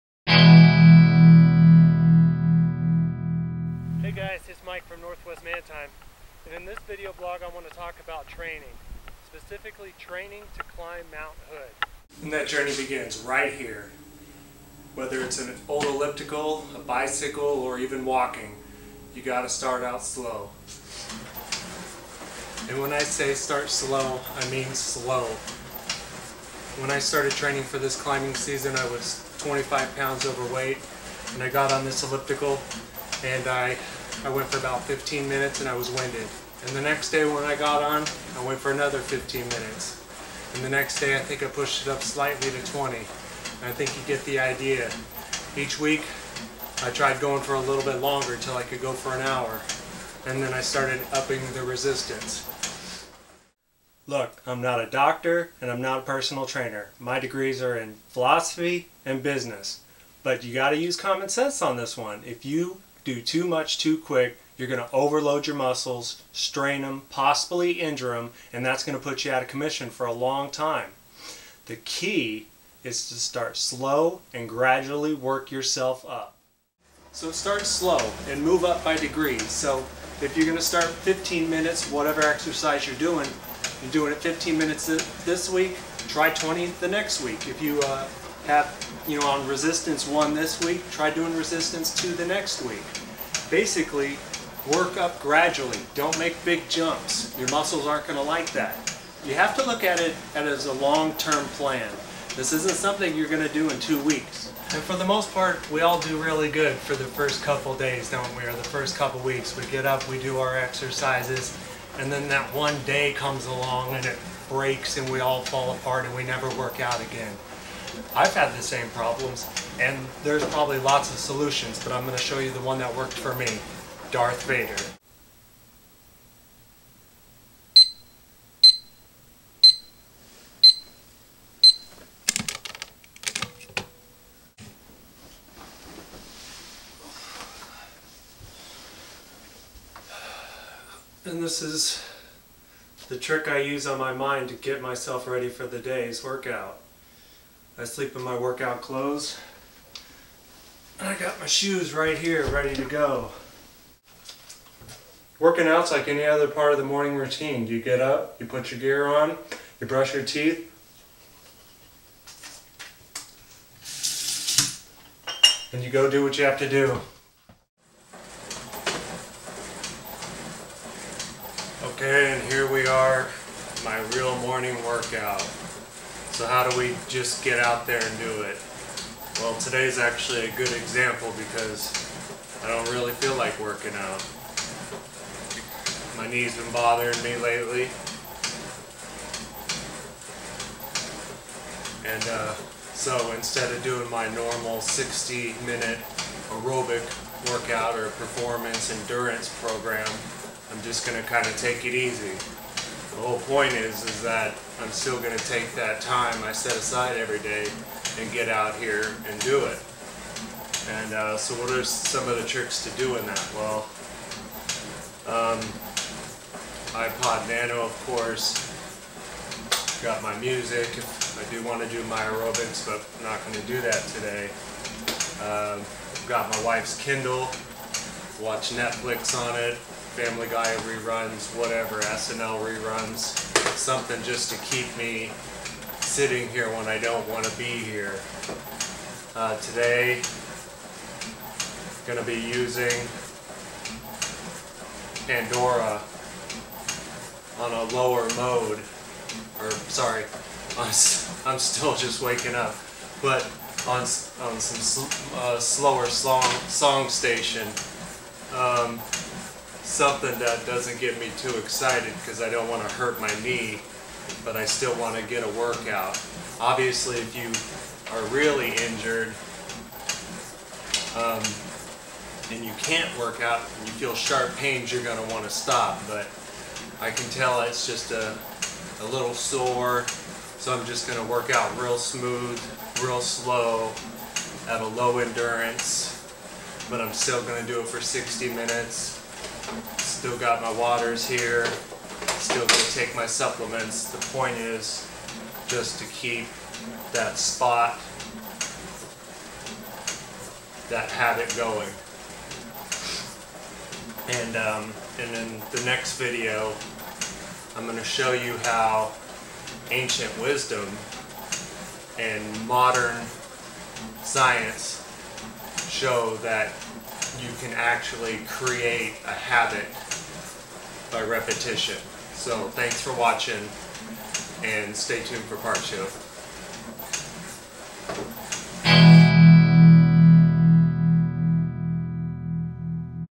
NW Man Time- The 1 Trick You Need to Get Into Shape for Mountaineering
NW Man Time- Climbing to the top of Mount Hood is not as easy as you've heard: you need to train. This is part one of two video that will get your butt off the couch and prepare you to summit the world's most climbed mountain, Mount Hood.